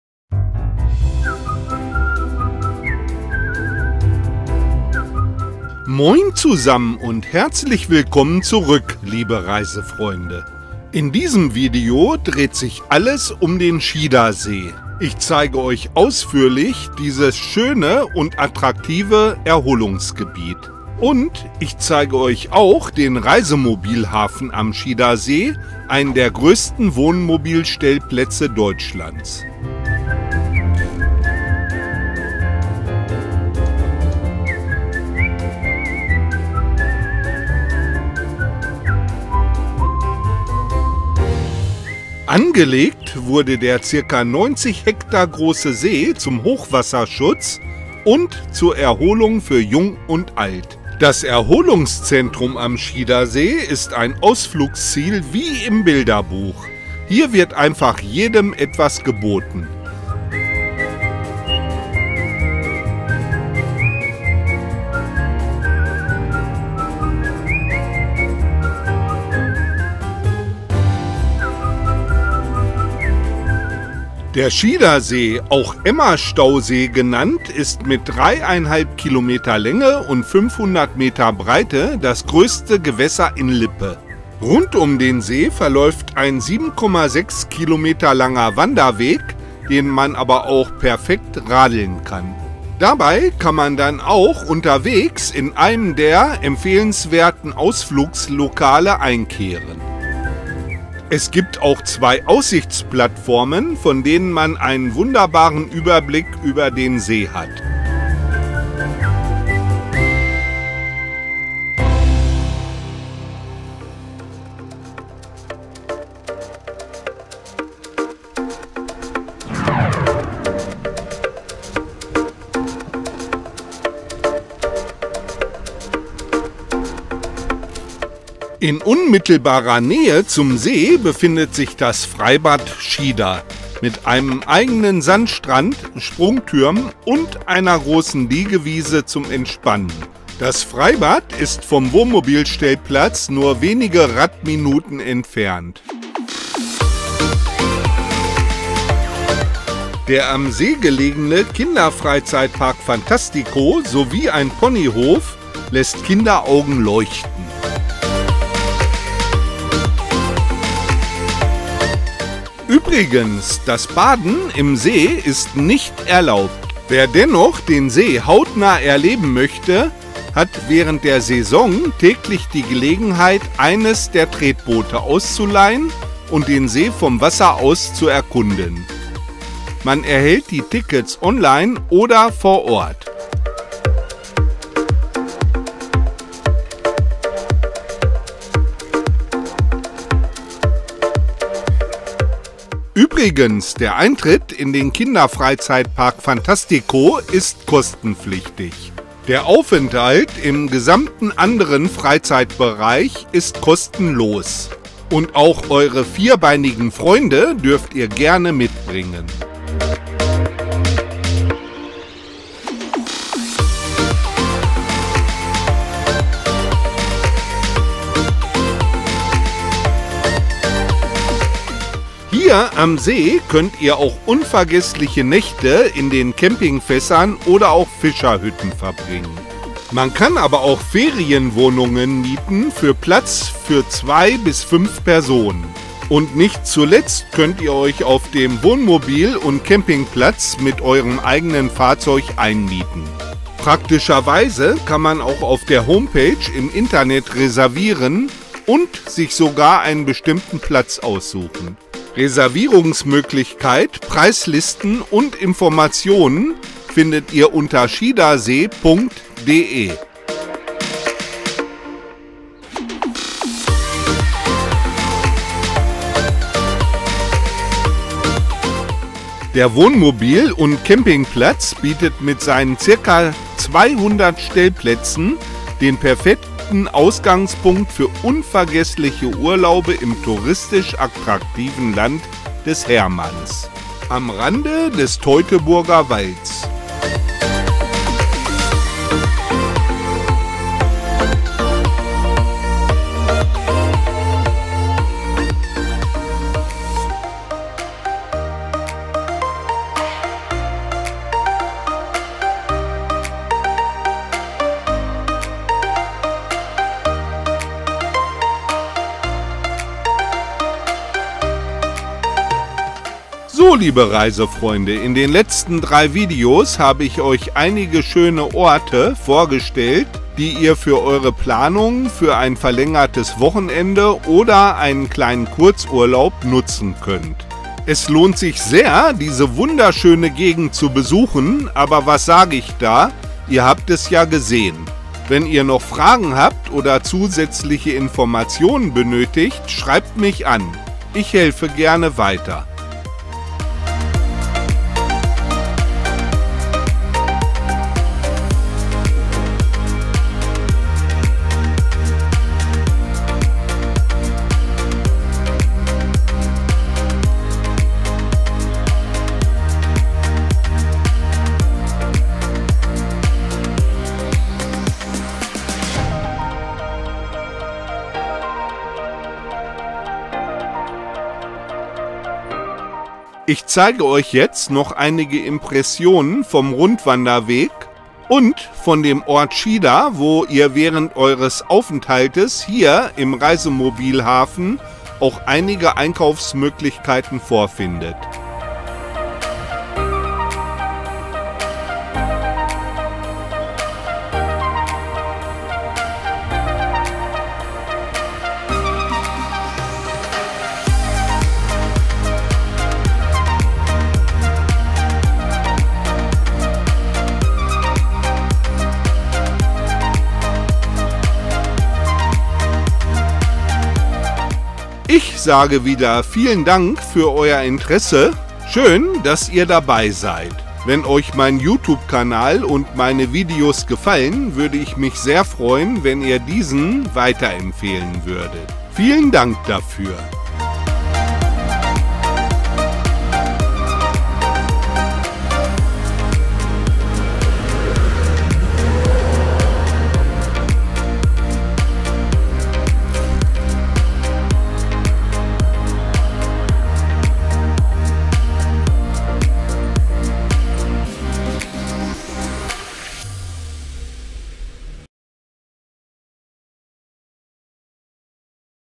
Wohnmobilhafen 🇩🇪& Camping Schieder See. WOMO 🚐Tour zum Nachfahren, Stellplätze & Ausflugtipps
Im dritten Teil meiner kleinen Videoreihe, Bad Pyrmont, Lügde und Schieder See nehme ich euch mit an den See. Ich zeige euch das schöne Freizeit & Erholungsgebiet und den Reisemobilhafen.